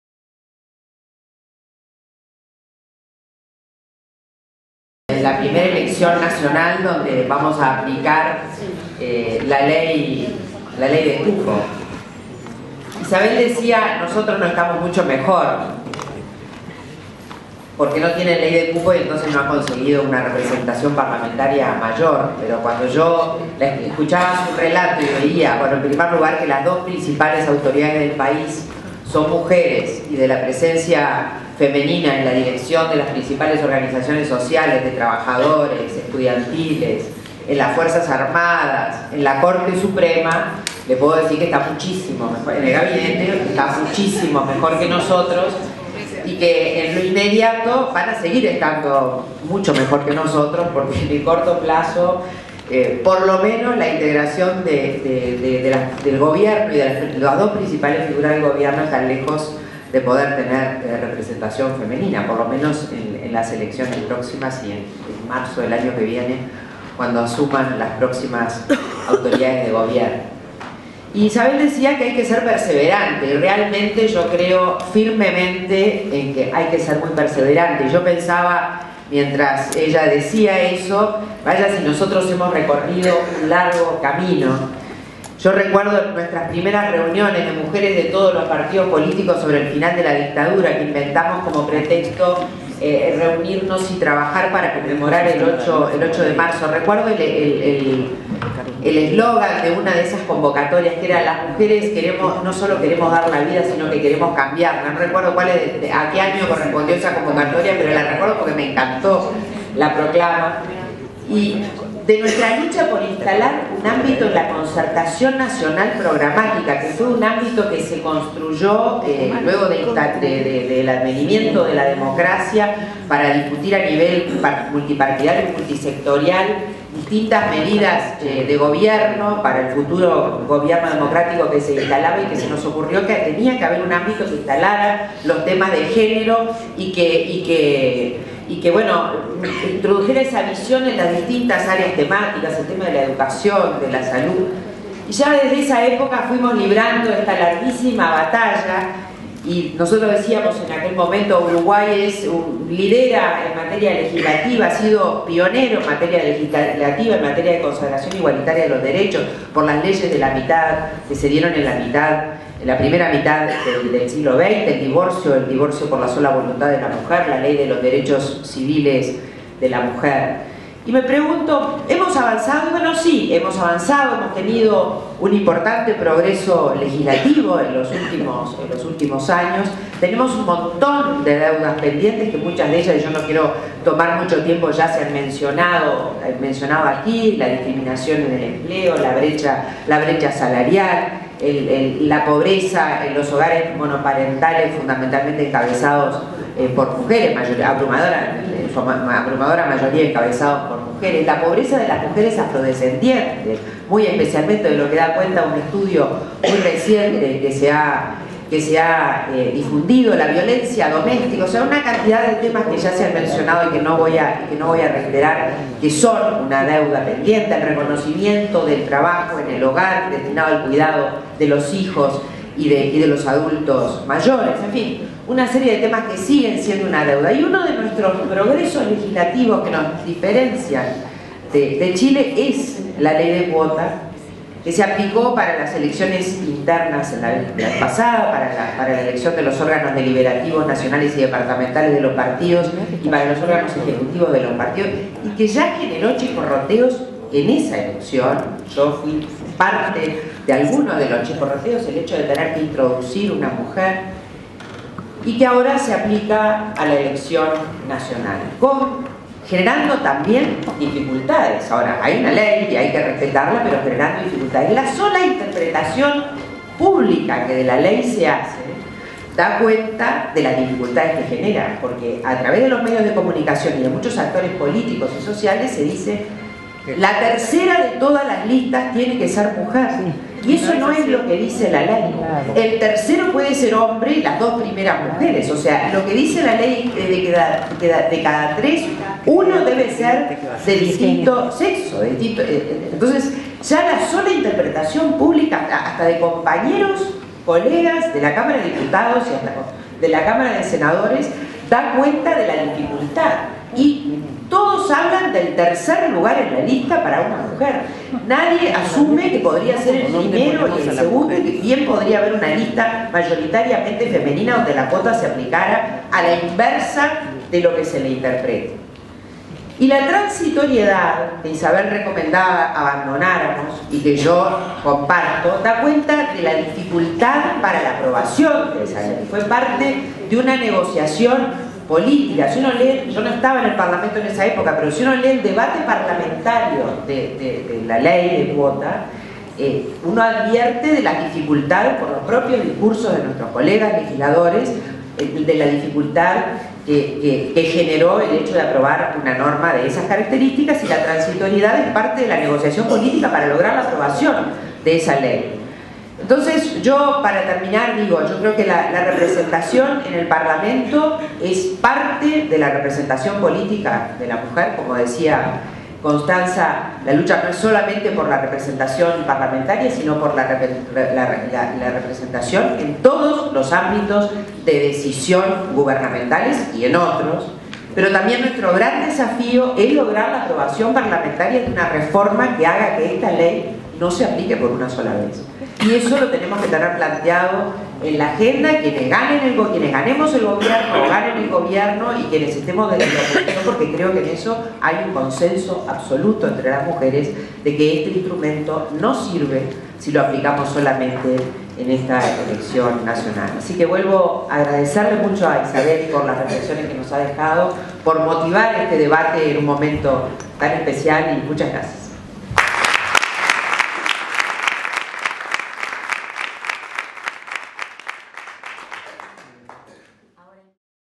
DIALOGOS A LA PAR - Ana Lía Piñeyrua, 25 de julio de 2014
DIÁLOGOS A LA PAR Participación política de las mujeres: De las cuotas a la paridad

2014 es un año electoral en Uruguay en que por primera vez se aplica una leydecuotas que garantiza al menos un 30% de mujeres en las ternas de las listas.

La Oficina Regional Suramérica del Instituto Interamericano de Derechos Humanos (IIDH), en conjunto con la Bancada Bicameral Femenina del Parlamento del Uruguay, Cotidiano Mujer y la Fundación Salvador Allende, convocaron a un diálogo con la Presidenta del Senado de Chile, Isabel Allende Bussi.

25 de julio de 2014, Montevideo, en la Antesala del Senado de la República Zelmar Michelini